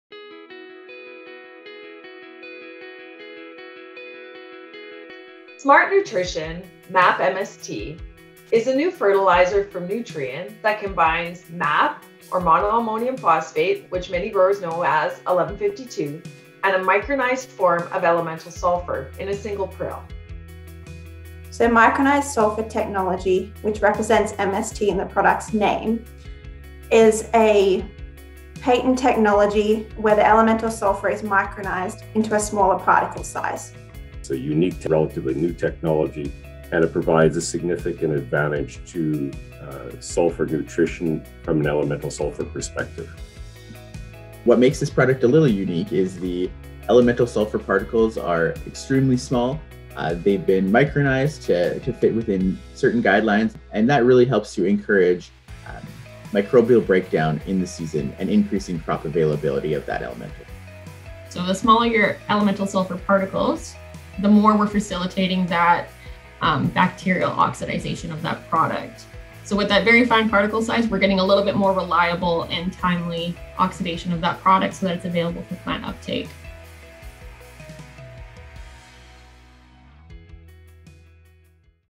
What is Micronized Sulfur Technology?
Representatives from Nutrien Ag Solutions across Western Canada share how the micronized sulfur technology used to create Smart Nutrition MAP+MST impacts elemental sulfur oxidation for increased nutrient availability.

Experience the latest, most efficient system for delivering sulfur and phosphate to meet your crop’s needs with Smart Nutrition™ MAP+MST®.